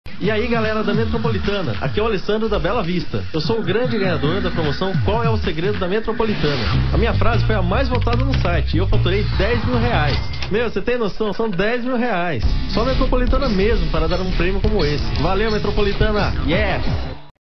Qual o Sergredo da Metropolitana - vinheta com Alessandro Tanner, ganhador do prêmio de R$ 10.000,00
Gravação da vinheta de apresentação de Alessandro Tanner, ganhador do prêmio de R$ 10.000,00 da promoção Qual o Segredo da Metropolitana em 2006.